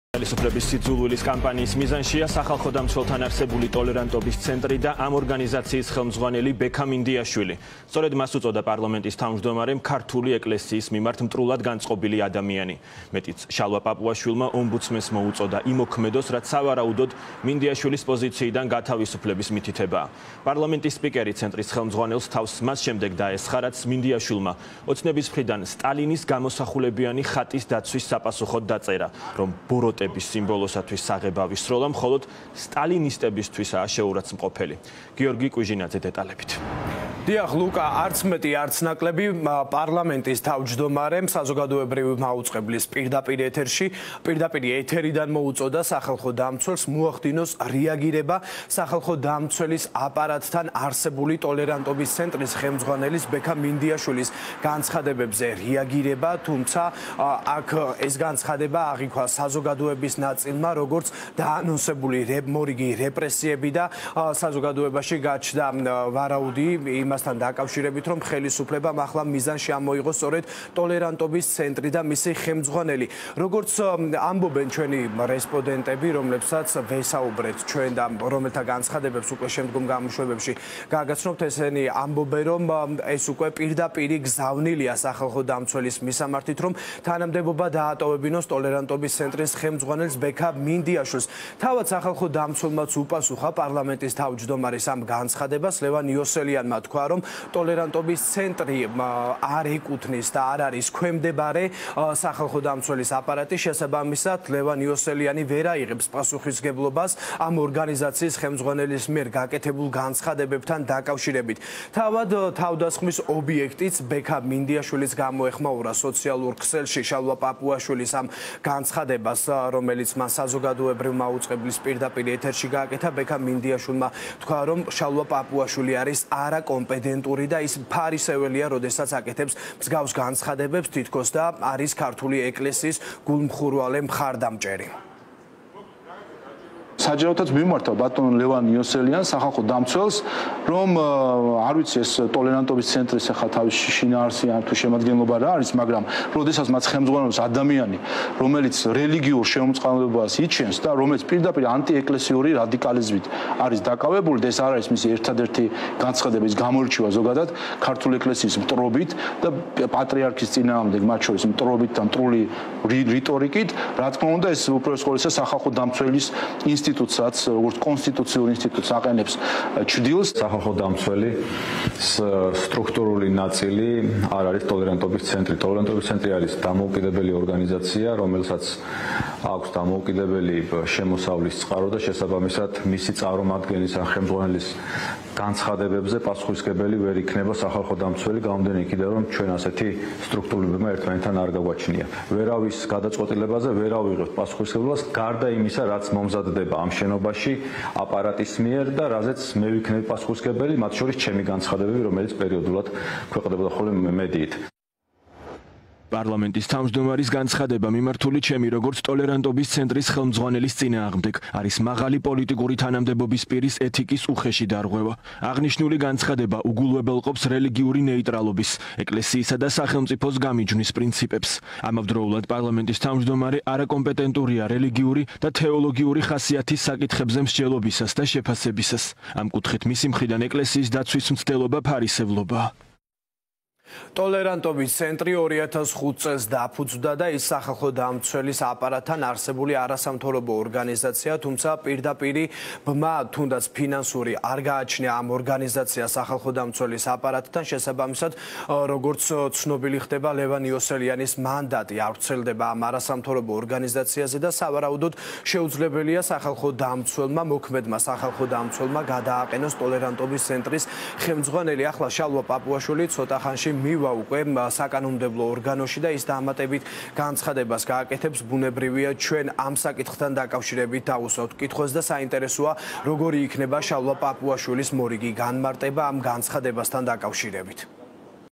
ტოლერანტობის ცენტრი — სიძულვილის სახელისუფლებო კამპანიის ახალი ობიექტი
▪️ ხელისუფლების სიძულვილის კამპანიის ახალი ობიექტი სახალხო დამცველთან არსებული „ტოლერანტობის ცენტრია“